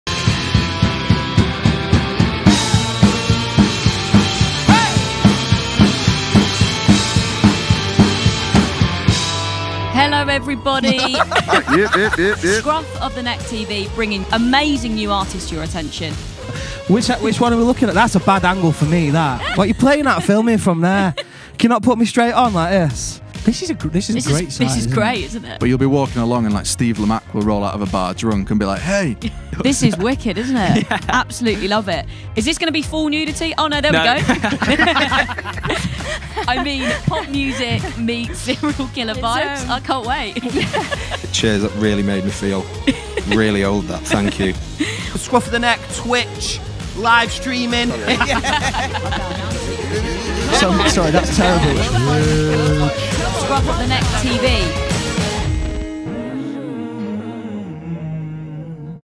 FEED Partner - Scruff Of The Neck TV boosts new bands (Blackmagic Design)
Manchester UK-based music company Scruff Of The Neck, launched Scruff Of The Neck TV this year to bring new bands to new audiences. The channel was built using gear from Blackmagic Design